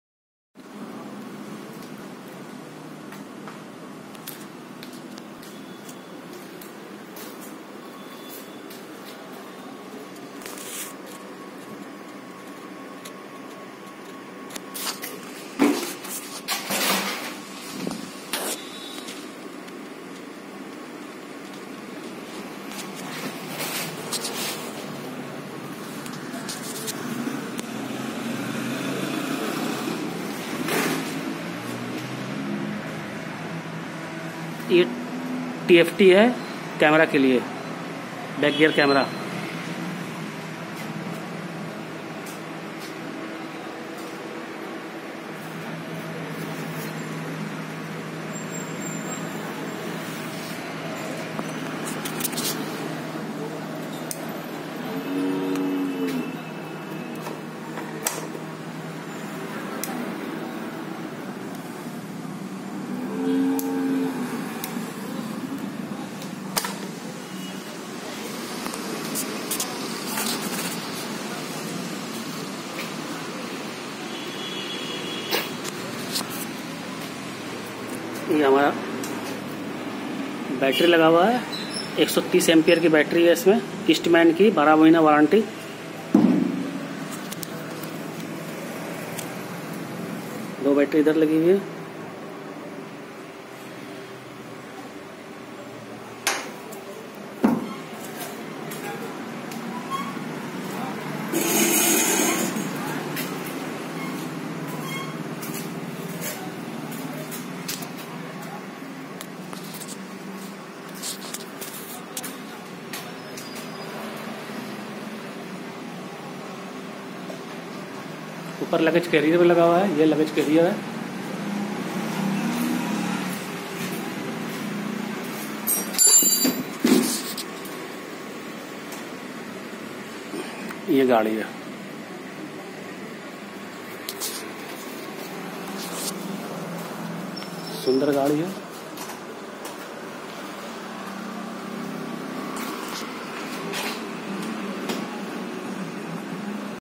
MAYURI Grand (top model)
MAYURI Grand (top model) is from SEARA Electric  with 1200 watt motor and heavy controller it is with fan, tape with Bluetooth, center light for collection of money in night and back gear camera with  LCD SCREEN & 130 Ah Eastman battery 12 month warranty..